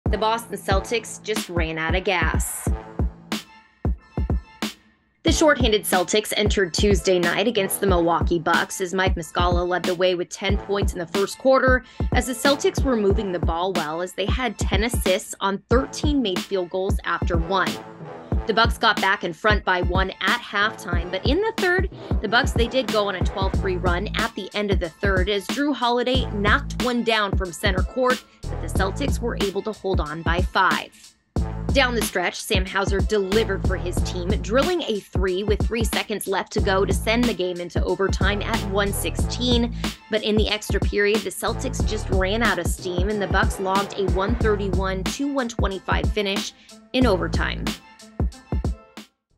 2/14 Putnam Postgame Report: Running Out of Steam
The shorthanded Celtics nearly took down the Bucks, as Sam Hauser sent the game into overtime, but Giannis and the Bucks pulled back in front with a 131-125 finish.